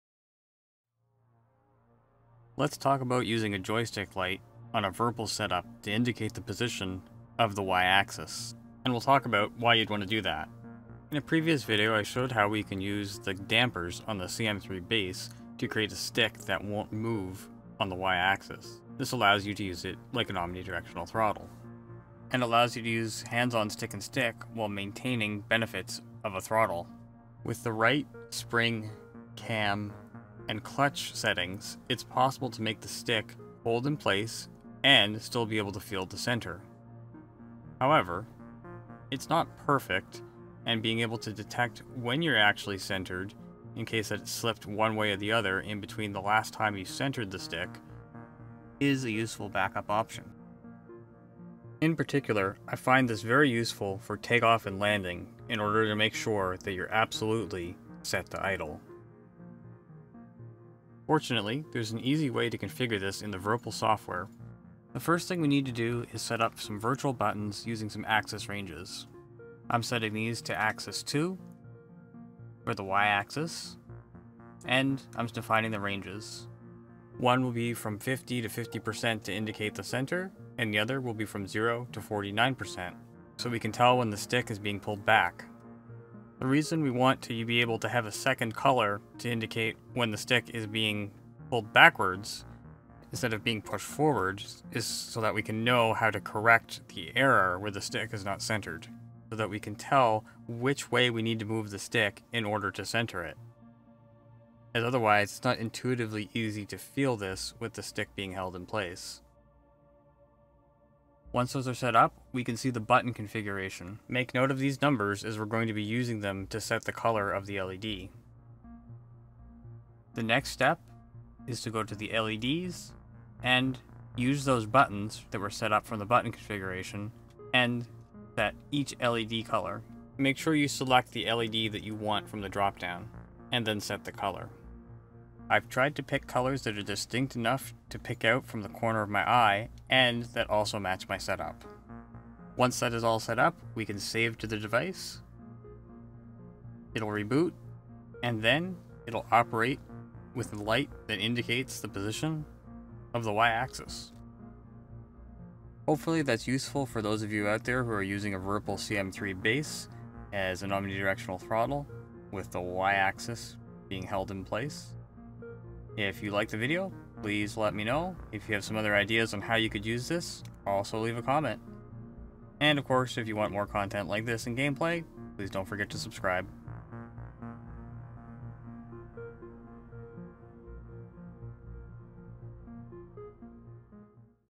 Virpil Y-Axis Position LED Indicator Tutorial VPC Software | CM3 Base Omnidirectional Throttle HOSAS
Using the VPC software to set a LED light that indicates the position of the Y-Axis for an omnidirectional throttle.